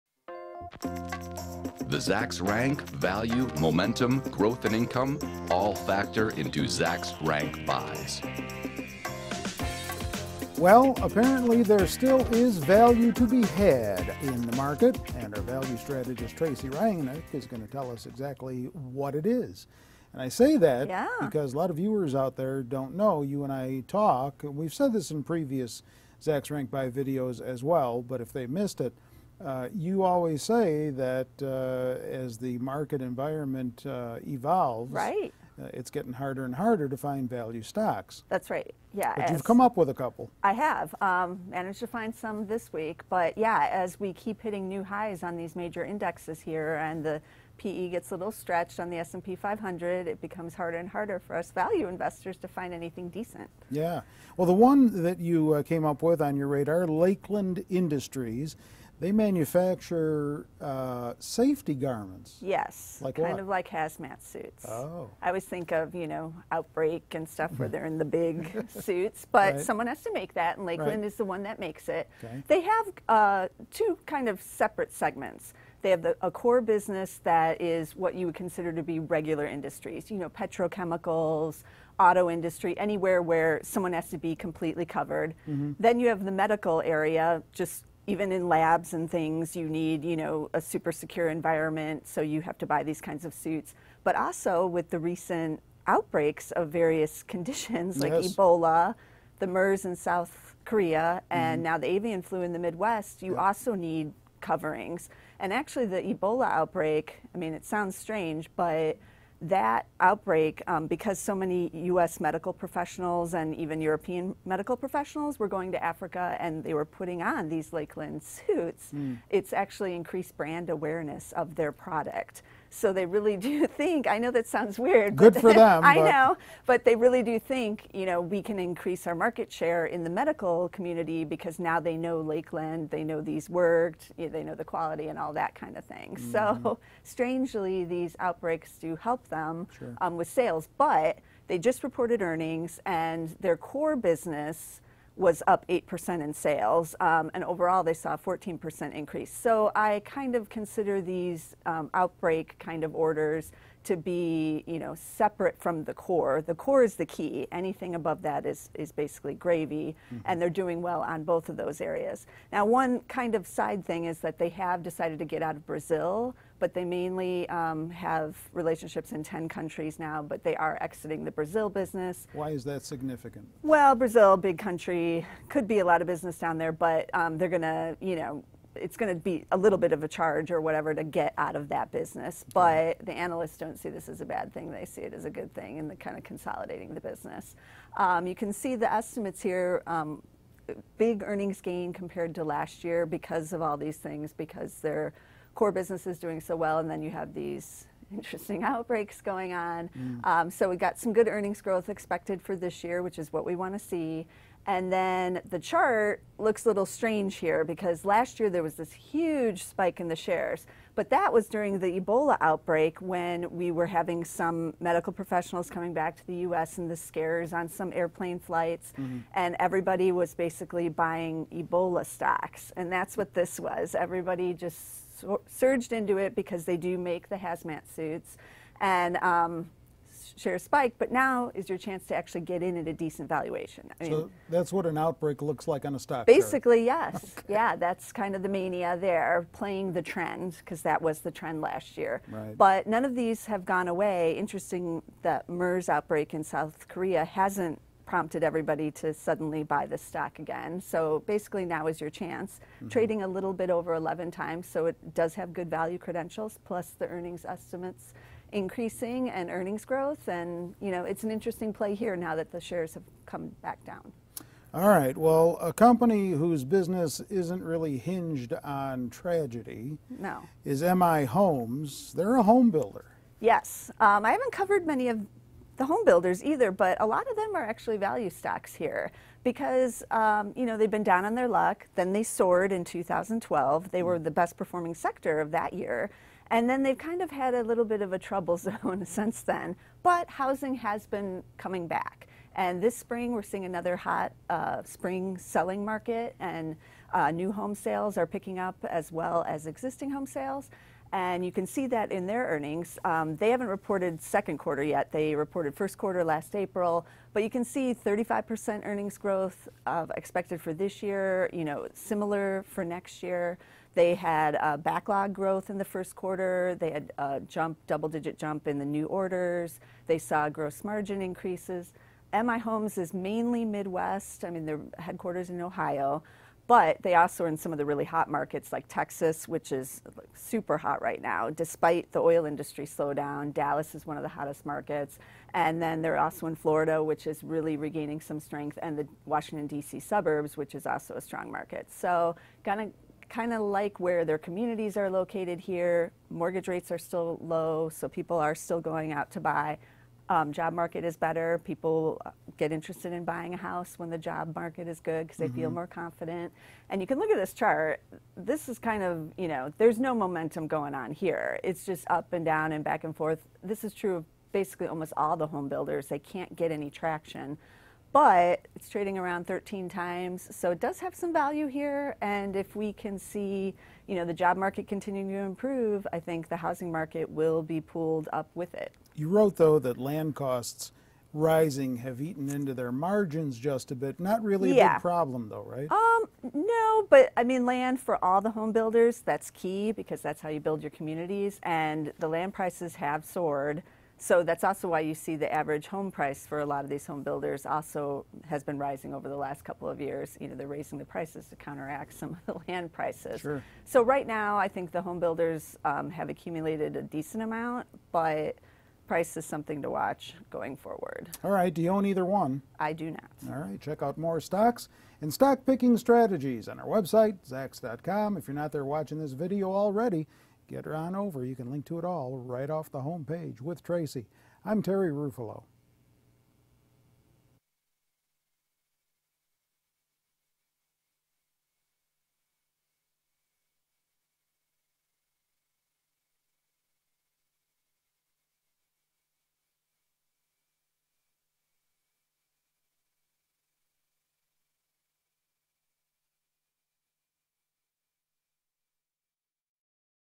This Week’s Best Value Stocks: (LAKE) (MHO)
Tracey Ryniec, Stock Strategist at Zacks Investment Research, discusses two stocks that might be worth a closer look by investors: Lakeland Industries (LAKE) and M/I Homes (MHO).

Both companies are expected to deliver big earnings growth in 2015 with Lakeland expected to see a 212% jump and M/I Homes a 35% gain.  And both are trading well under the average of the S&P 500, which is rare in this booming stock market.

But what else should investors take away from these two companies? Watch our short video below to learn more about these value stocks!